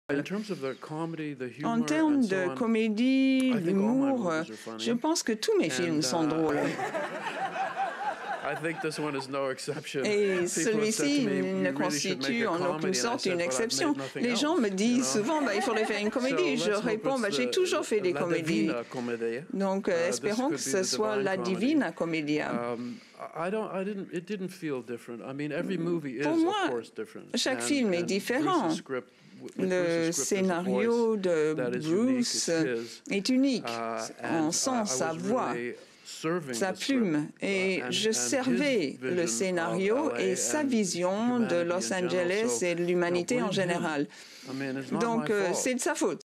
Cannes 2014 : David Cronenberg : "Je pense que tous mes films sont drôles"
David Cronenberg ne voit pas tous ses films comme des drames et il explique pourquoi.